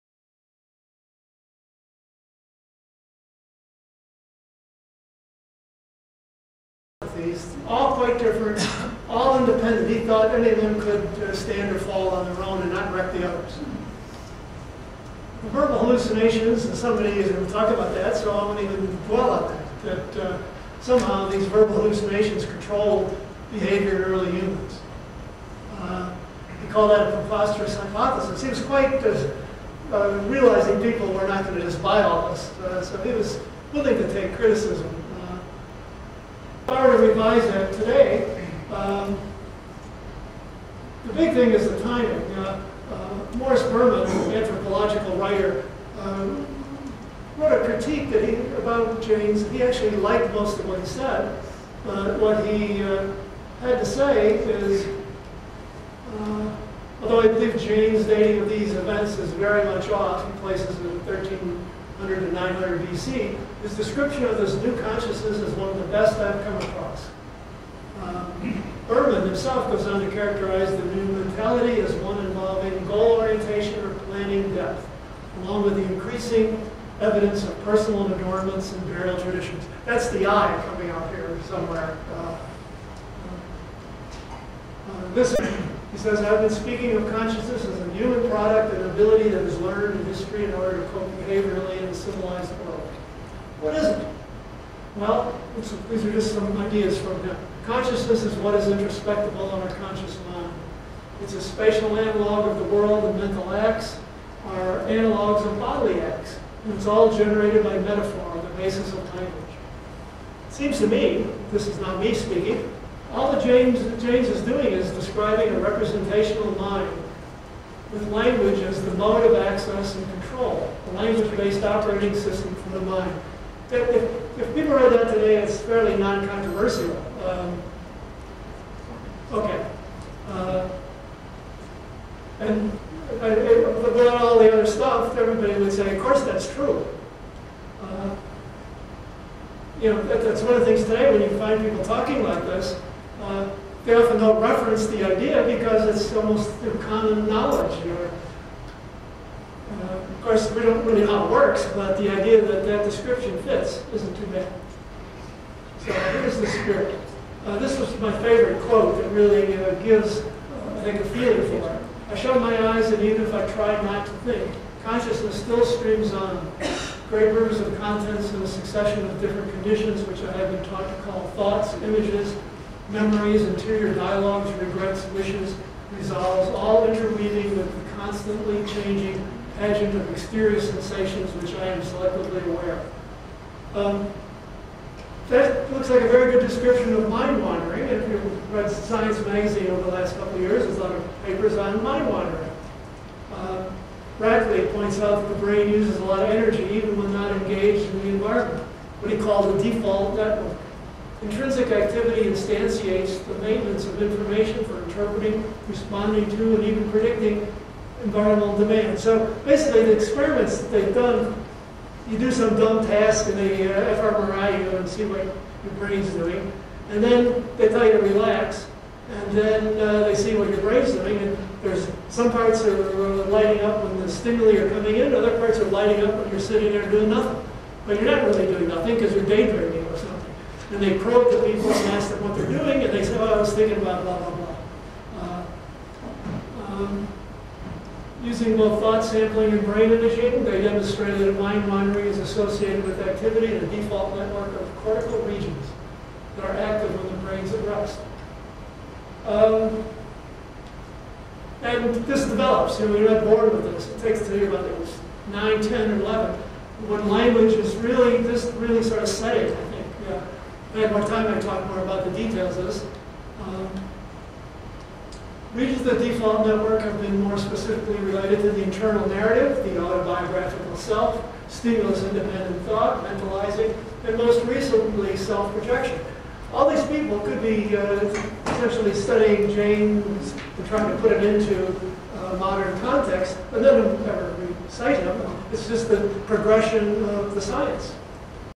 Consciousness is Just A Word [Language and Julian Jaynes's Theory of Consciousness] | John Limber
"Consciousness is Just A Word," a lecture by Professor of Psychology John Limber.

Highlights from the Julian Jaynes Session at the 2008 "Toward A Science of Consciousness" Conference.  For more on Julian Jaynes's theory, including full length lectures and interviews, please

Read Professor Limber's chapter "Language and Consciousness" in the book "Reflections on the Dawn of Consciousness: Julian Jaynes's Bicameral Mind Theory Revisited"